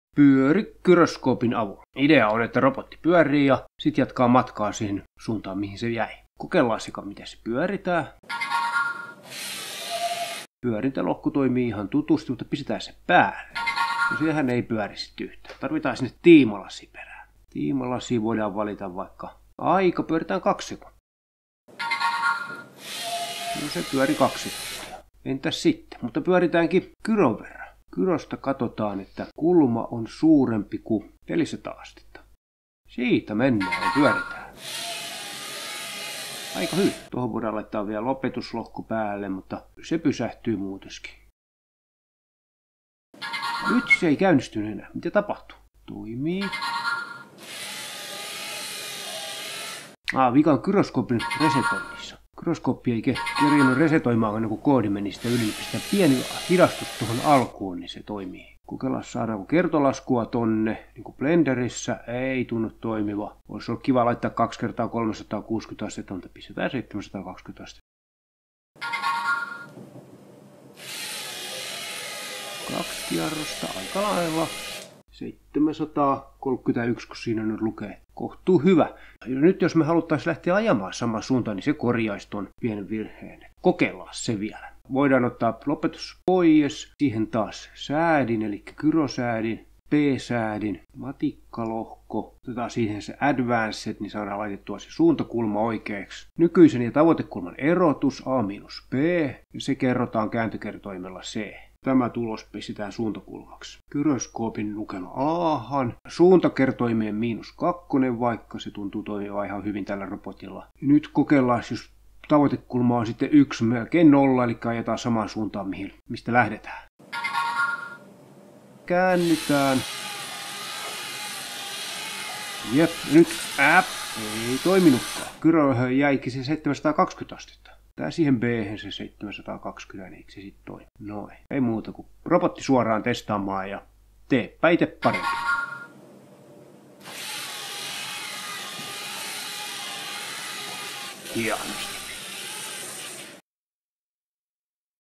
Robottitanssi 3: Pyöri ja jatka suoraan gyrolla
Spin and drive straight using gyroscope. LEGO Mindstorms Ev3.

Spinning (or pirouette) is an important element in dance. It works very reliably using EV3 robot.